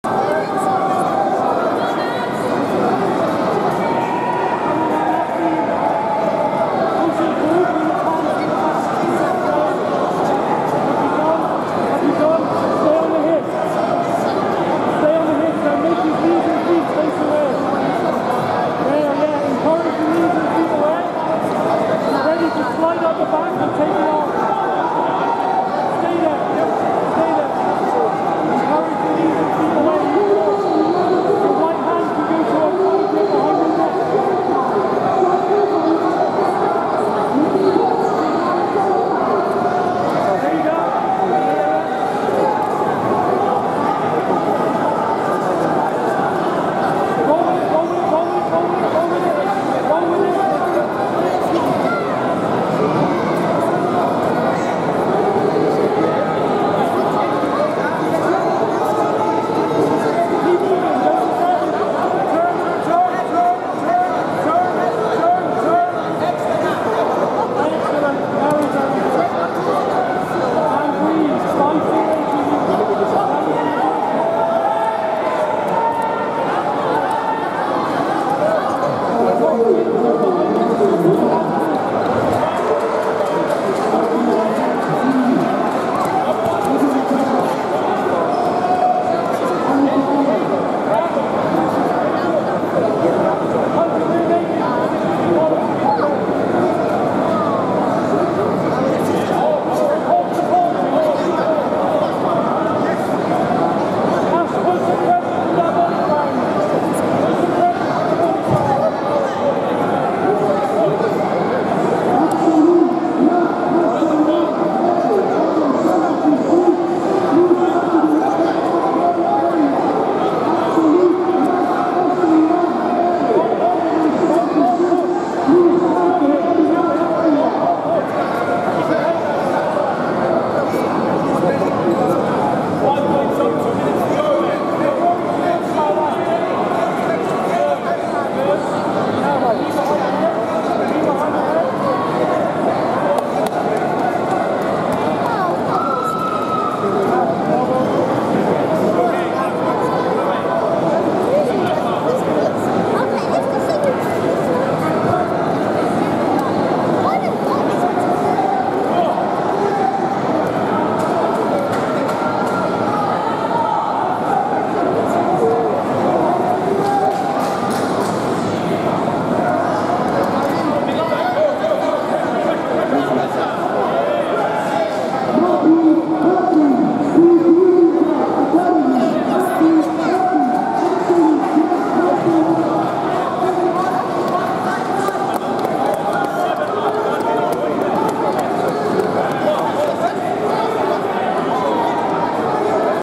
Ash Gibson vs Gary Bywater - All Stars BJJ Winter 2022 - Purple Adult - Open - Over 76kg
Ash Gibson (Gracie Barra Westbury) vs Gary Bywater (Viking Brazilian Jiu Jitsu)